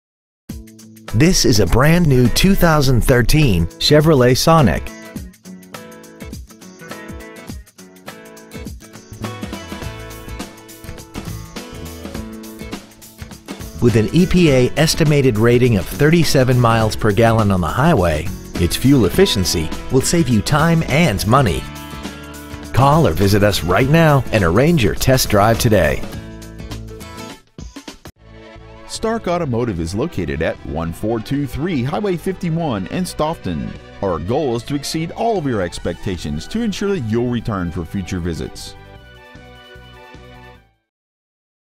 2013 Chevrolet Sonic Stoughton WI
Year : 2013
 Make : Chevrolet
 Model : Sonic
 Engine : 4 cyl., 1.4, turbocharged
 Trans . : automatic
 Exterior : maroon

 Interior : black
 Stock : 3130096